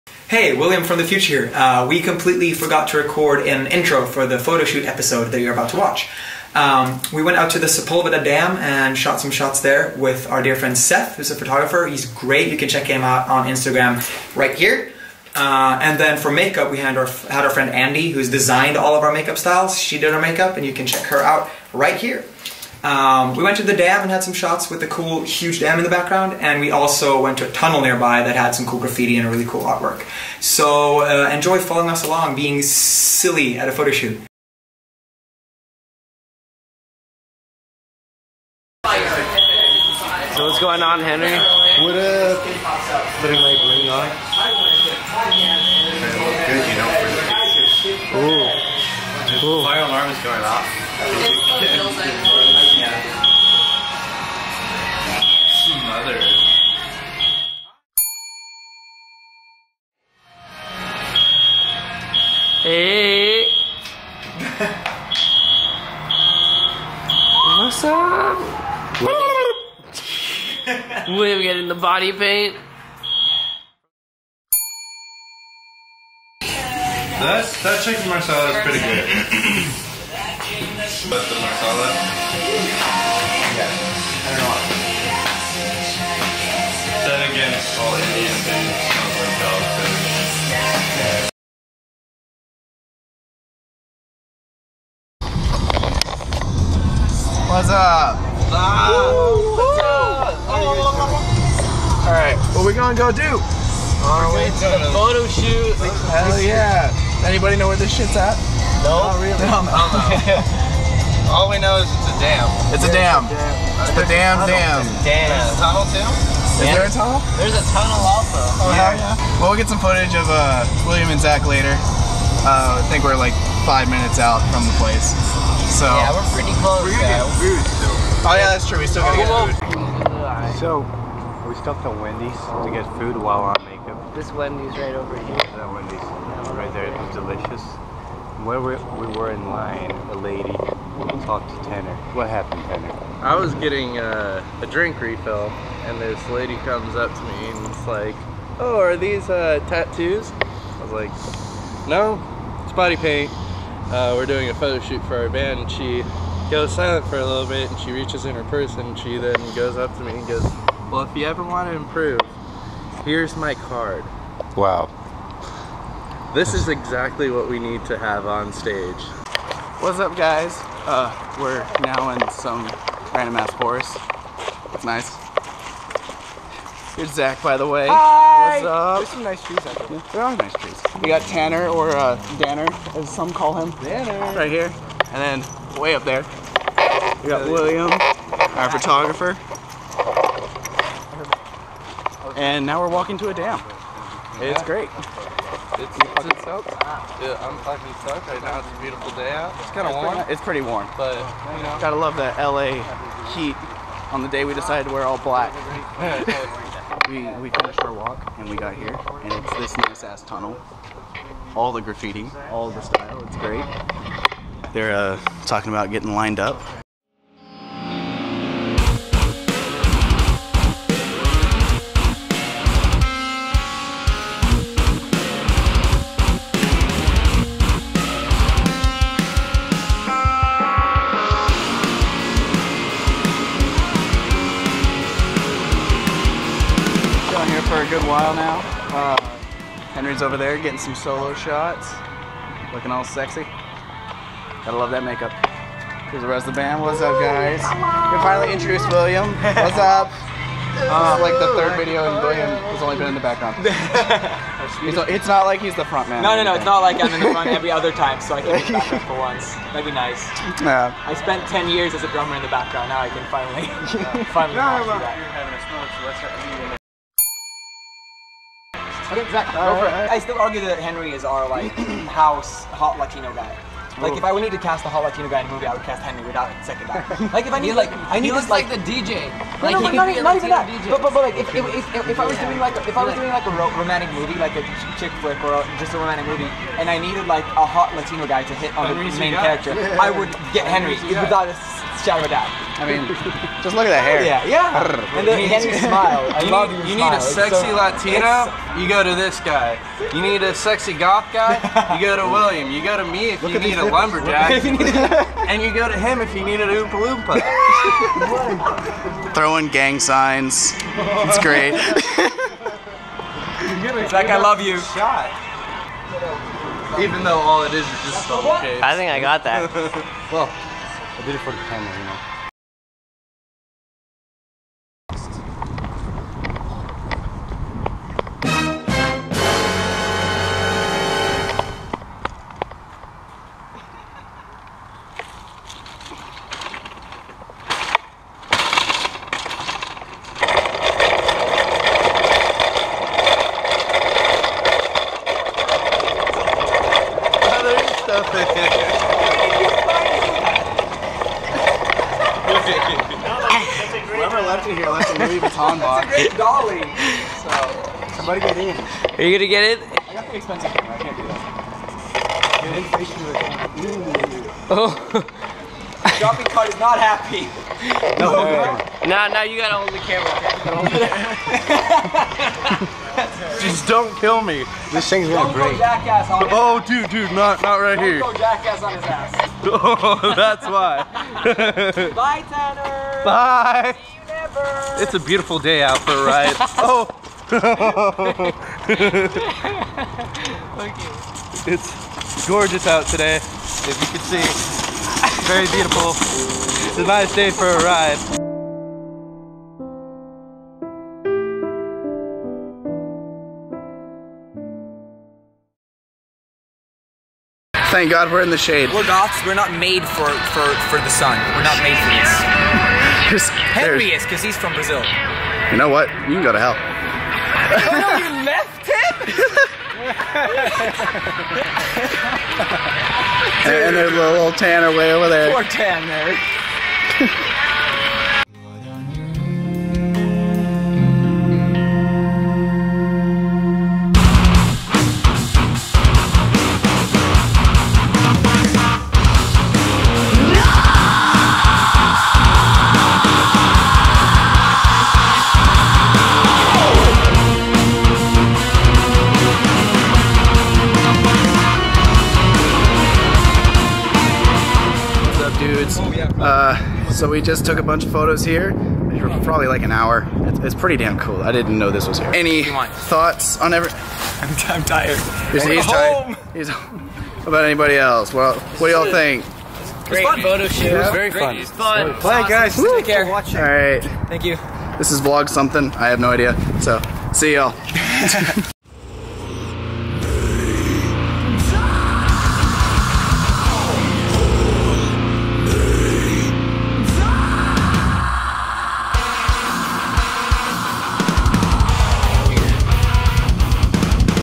Uncompliant Vlog Ep. 2 | The Damn Photoshoot
Uncompliant Vlog Ep.1 | Welcome to the Vlog!

In this episode of the Vlog we head out to a photoshoot with our dear friend Seth! We go the Sepulveda Dam(n) and have a great time. Tanner gets kidnapped and killed by a shopping cart, we try to look cool and a fire alarm is going of! Enjoy!